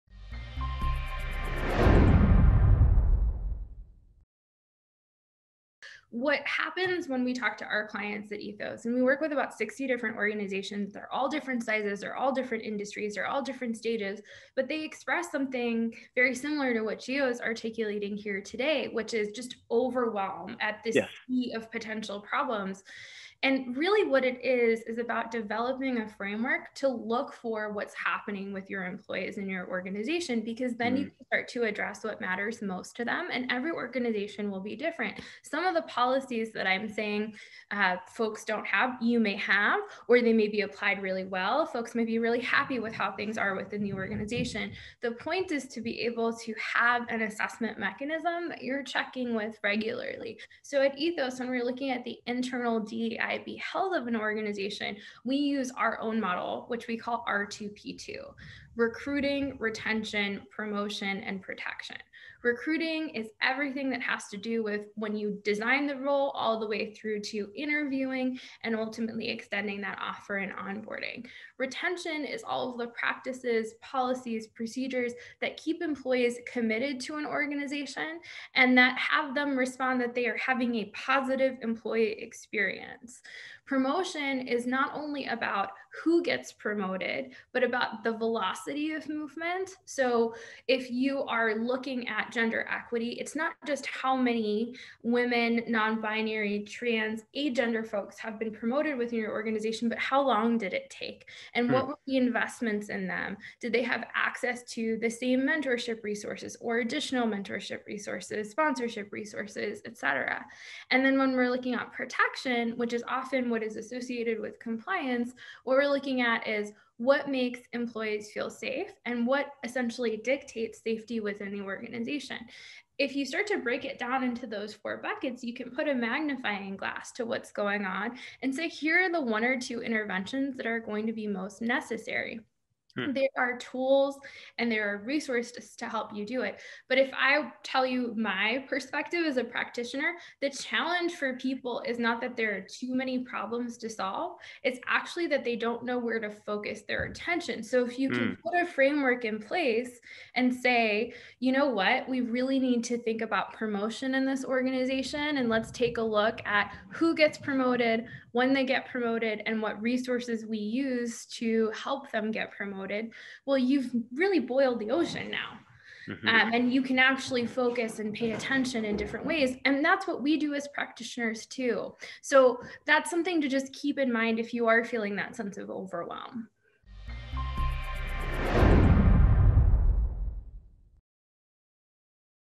Have an assessment mechanism that you are checking with regularly | Gender Equity Webinar Clip
Alida Miranda-Wolff, Ethos CEO, advises to develop a framework to look for what is happening with your employees in your organization so that you can start to address what matters most to them. Keep in mind that no organization will be the same! One way to start doing this is by implementing an assessment mechanism that you are checking with regularly.

ComplianceLine CEO, Giovanni Gallo, sat in for a fireside chat with Ethos CEO, Alida Miranda-Wolff, on June 17th, 2021 to discuss the current climate within gender equity and how to truly build an inclusive organization that stands the test of time. During their one hour session, a range of topics were covered including:
How has the COVID crisis affected gender equity progress?
What are our goals as leaders in our organizations?
How do we achieve those goals?
What are some key data and benchmarks we can use to drive these initiatives forward?